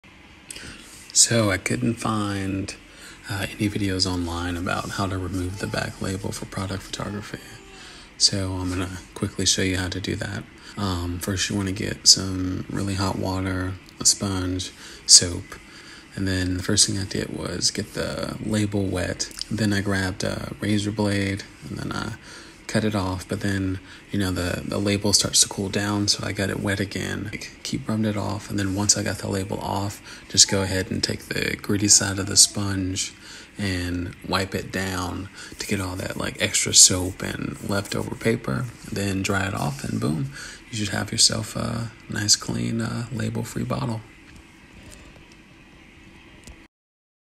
The easy way to remove, beer, whiskey, whisky, or wine bottle labels. (without emptying the bottle)
How to remove the label on whisky bottles for Photography.